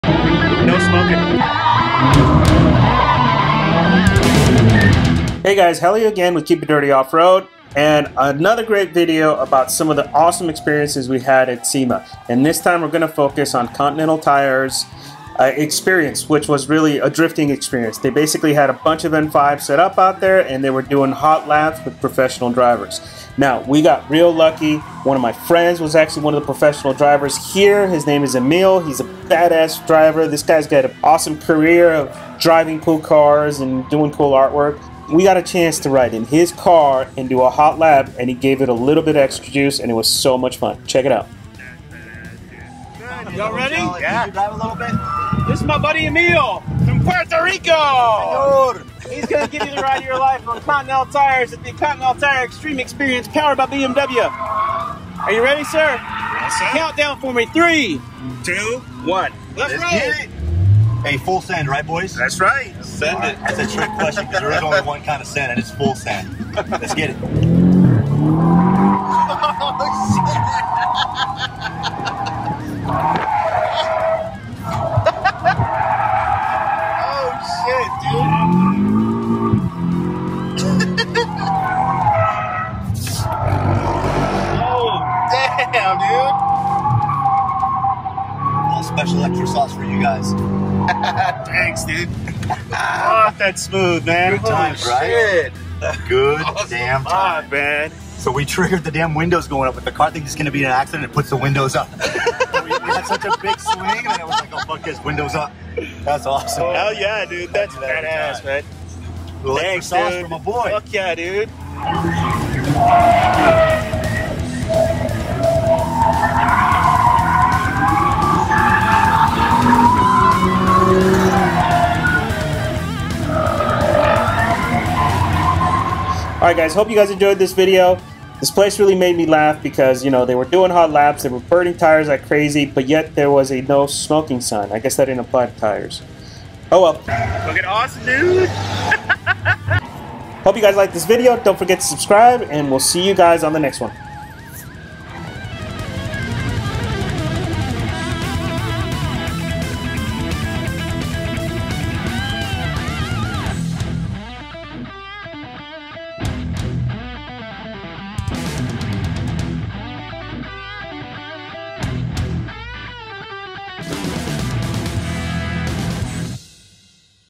Drifting BMW M5s - Continental Tire Extreme Experience SEMA 2019
One of the Best experiences at SEMA had to be taking a hot lap with my buddy Emile in a brand new BMW M5 drifting around a track for the Continental tire Extreme Experience.

This was in one of the corners of the show in the areas open to the public way in the back. It was a ton of fun and my buddy gave it a little extra sauce to make it way more fun.